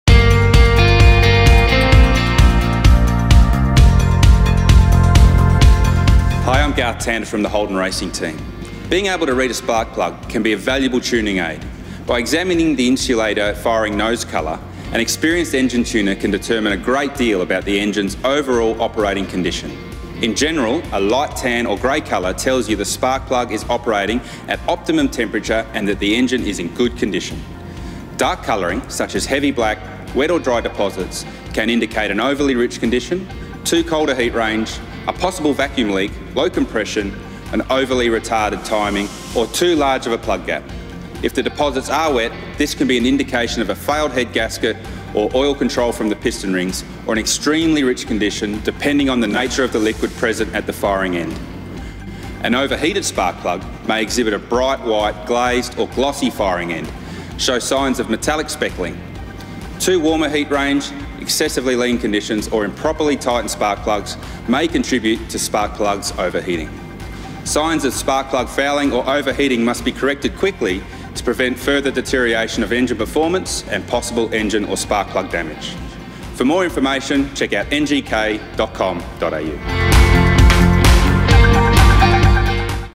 Garth Tander NGK Spark Plug Training - Reading a Spark Plug
Garth Tander from Holden Racing Team talks us through how to read a spark plug.